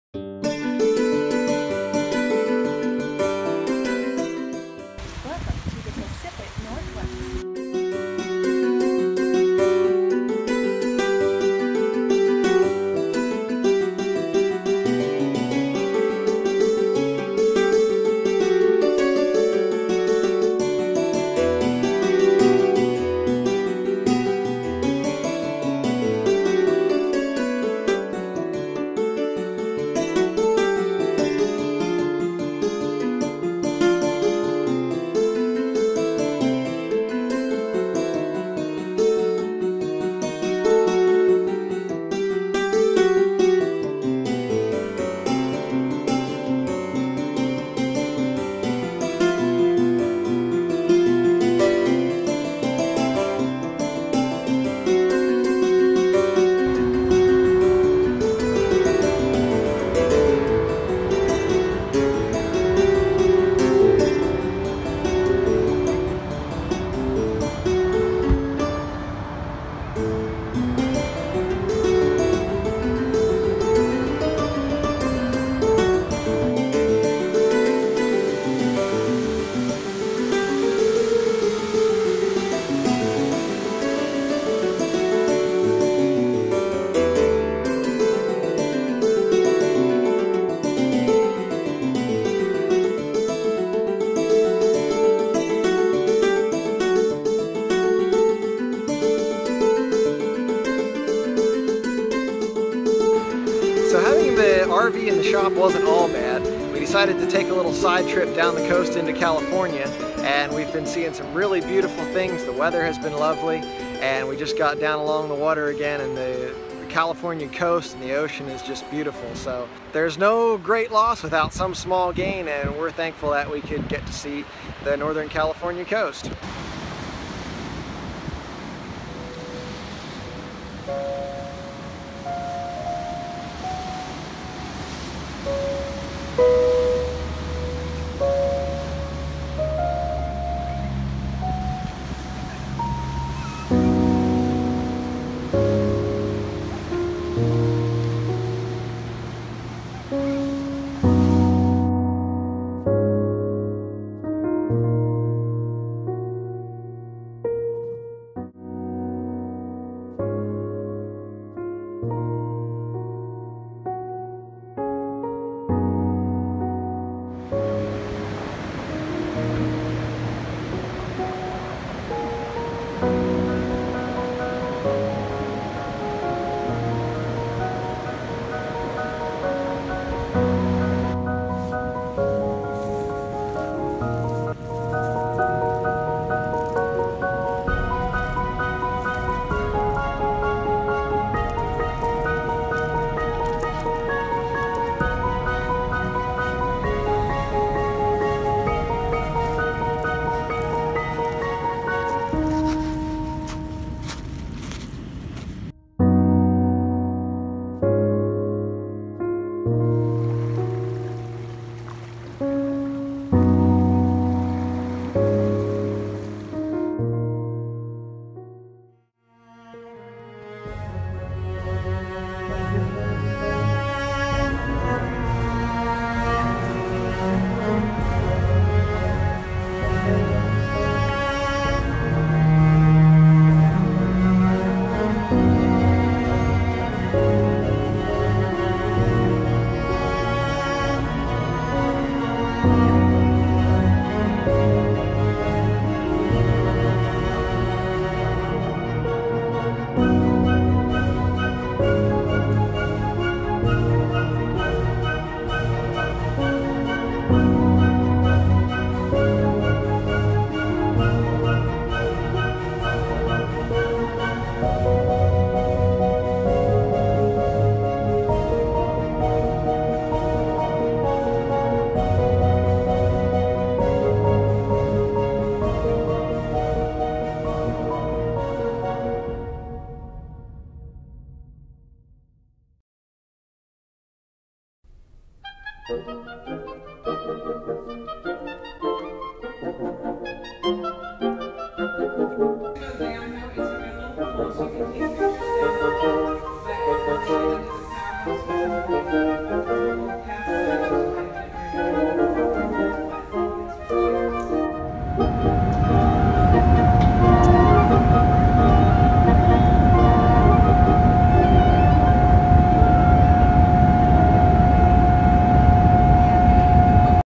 California
White our RV was being repaired, we took a side trip to see the redwoods of Northern California.